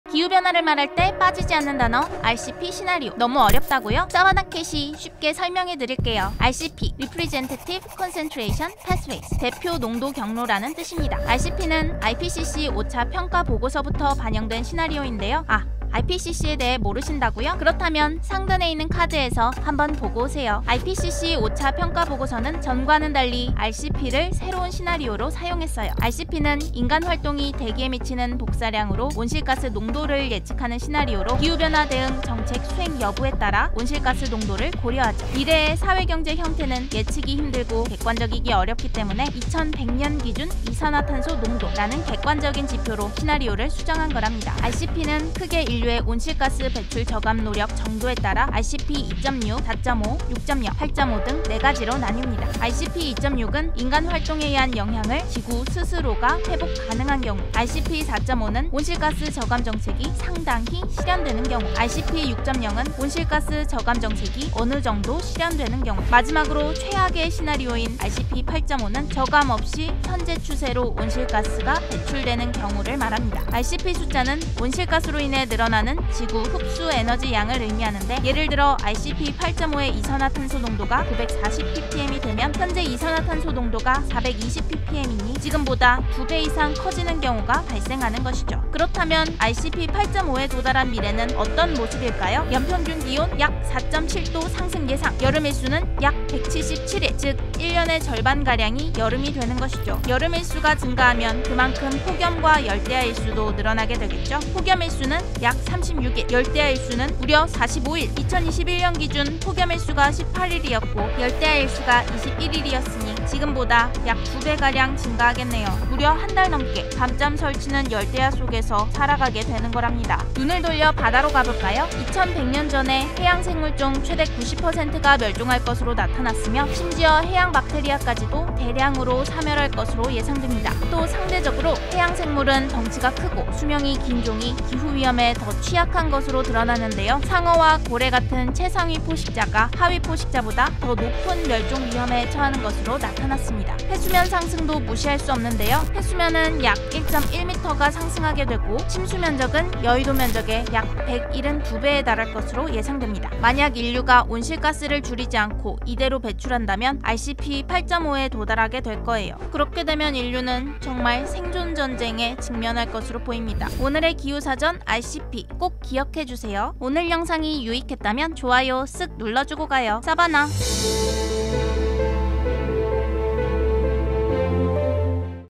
과학자들이 말하는 인류가 아무것도 하지 않으면 벌어지는 일들
오늘의 기후사전 : RCP (대표농도경로)

전 세계 과학자들이 2100년을 기준으로 분석한
기후 변화 시나리오!

그중에서도 전 세계 과학자들이 분석한
RCP 대표농도경로에 대한 이야기!

인간이 배출하는 온실가스 배출량에 따라서
지구와, 인류의 운명이 정해진다!

그중에서도 가장 최악의 시나리오는 RCP 8.5인데요,

인간이 온실가스를 줄이기 위한 노력을 하지 않는다면
2100년 인류가 겪을 미래는 말 그대로 재앙입니다.